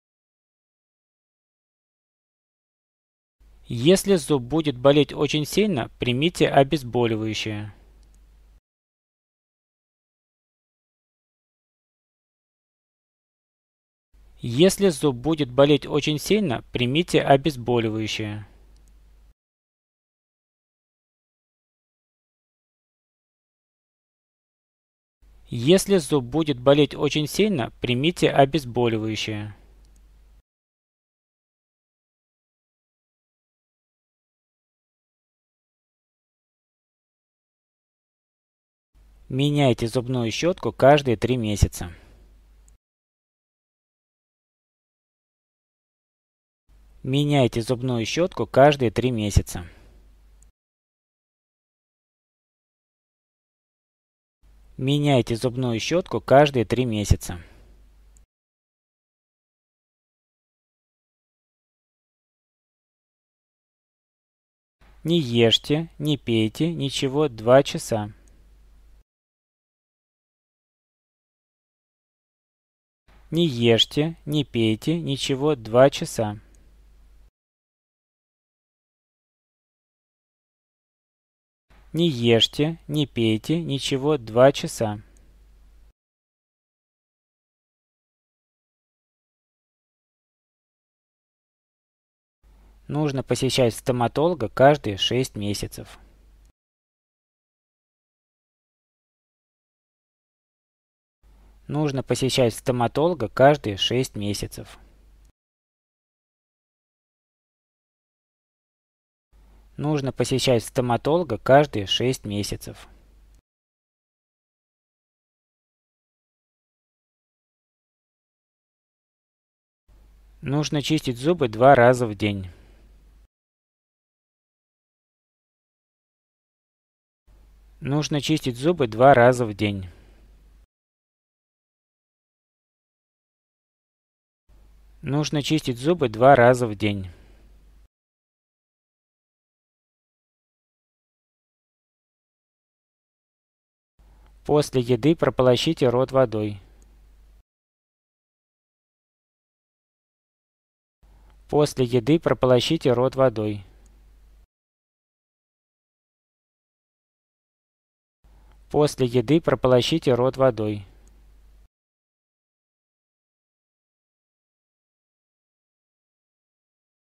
Russian Phrases and words / At the dentist in the clinic (376) / Russian language
Russian Phrases and words / At the dentist in the clinic / Russian language
Don’t eat or drink anything for 2 hours.
If you have a bad toothache, take a painkiller.
After having a meal, rinse your mouth out.
You should brush your teeth twice a day.
Change your toothbrush every three months.
You should visit а dentist every 6 months.